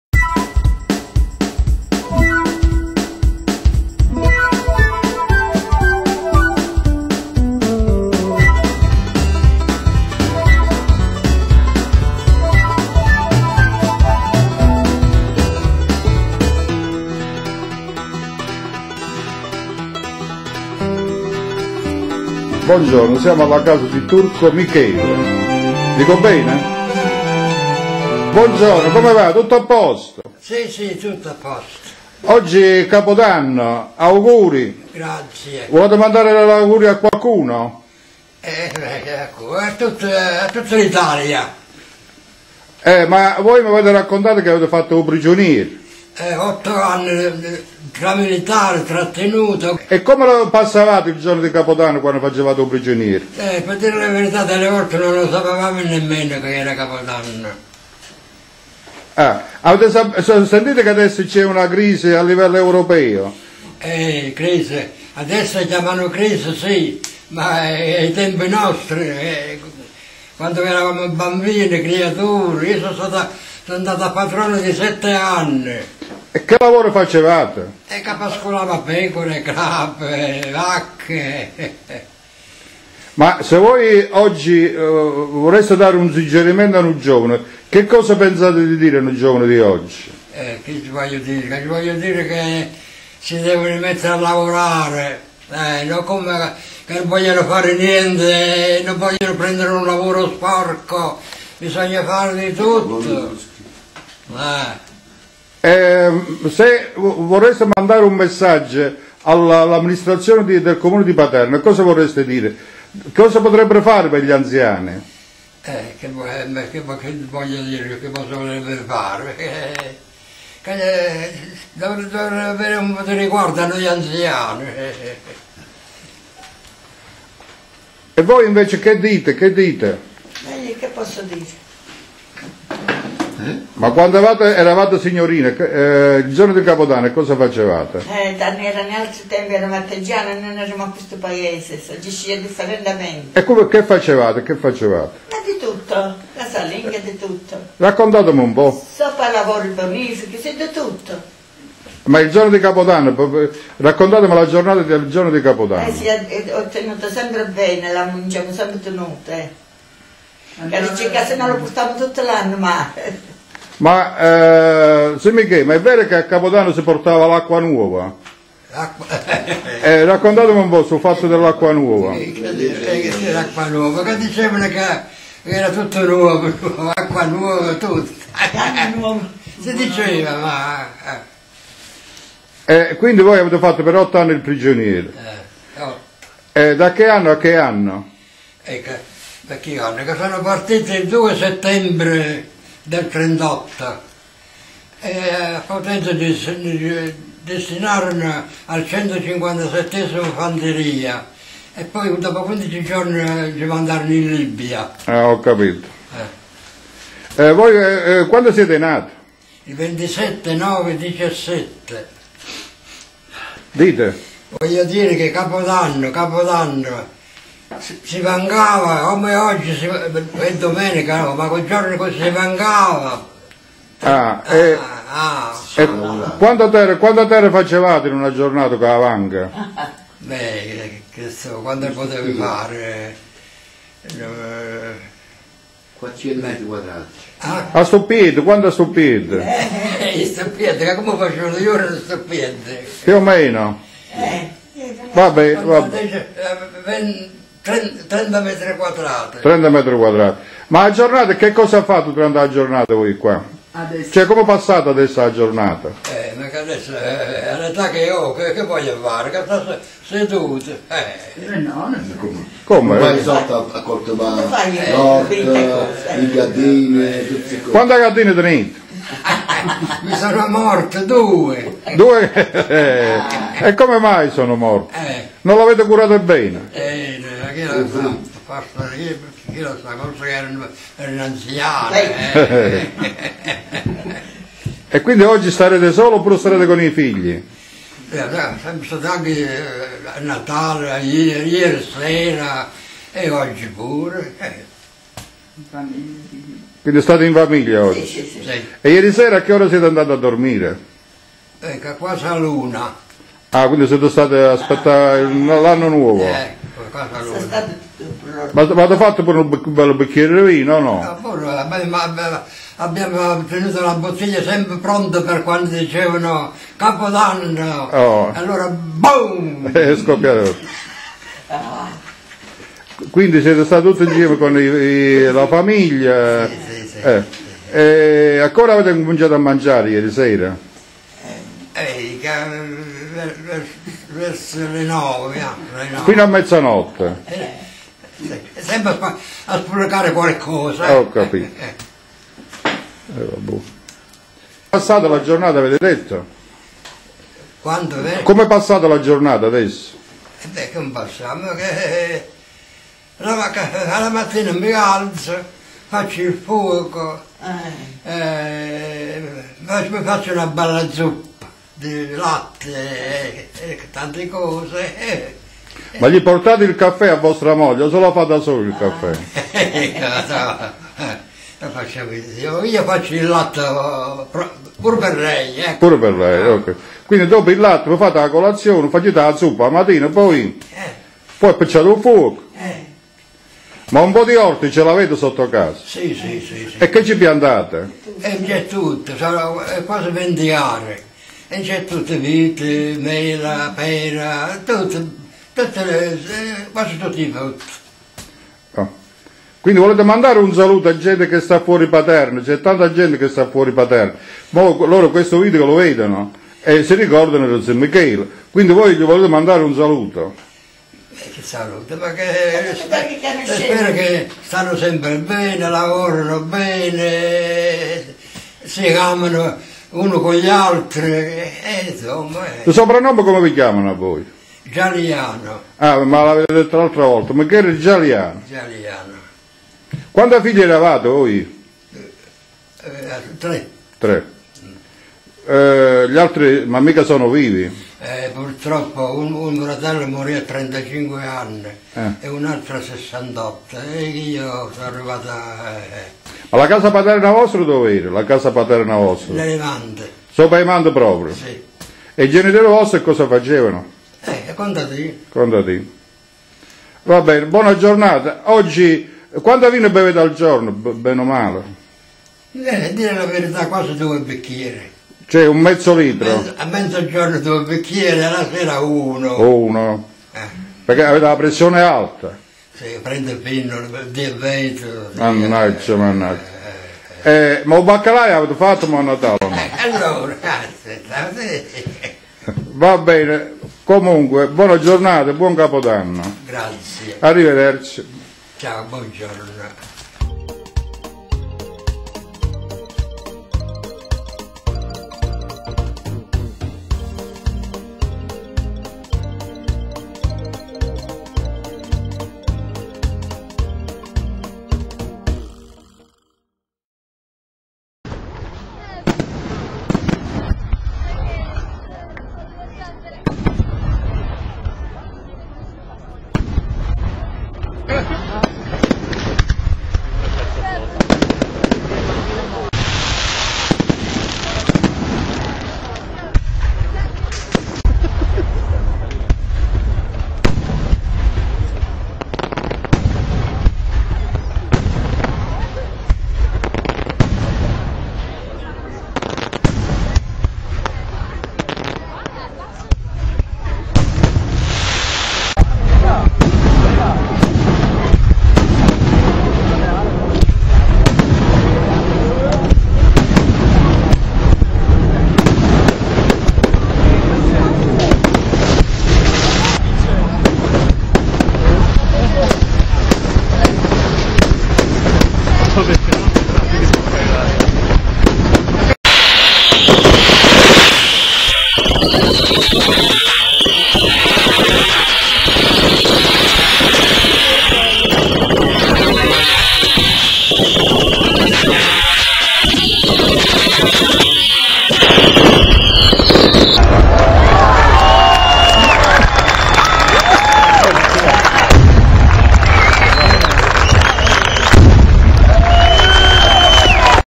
Auguri dal più giovane maschietto di Paterno
Turco Michele, persona simpaticissima,  il giorno del capodanno 2012  da gli auguri a tutti i paternesi sparsi nel mondo